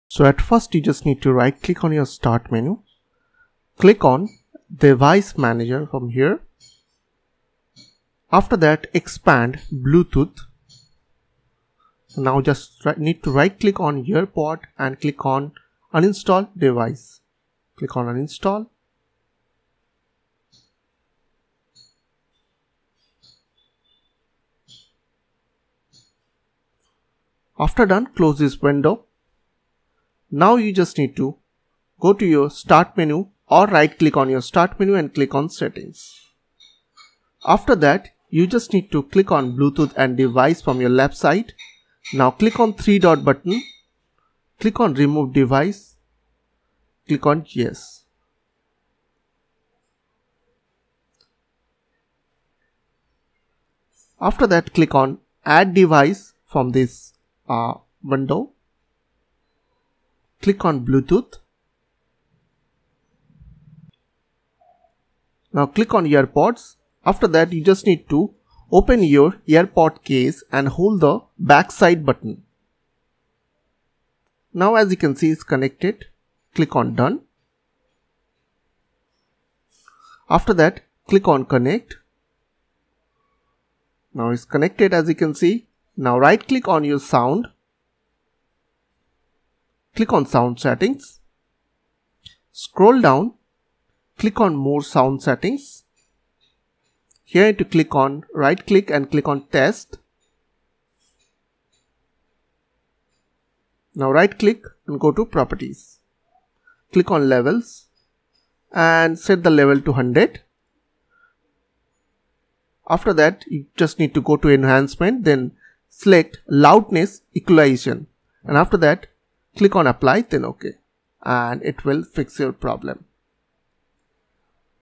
How to Fix Low AirPods Volume in Windows 11
Here in this video tutorial, I will show you guys how to fix low AirPods volume on Windows 11 PC or laptops.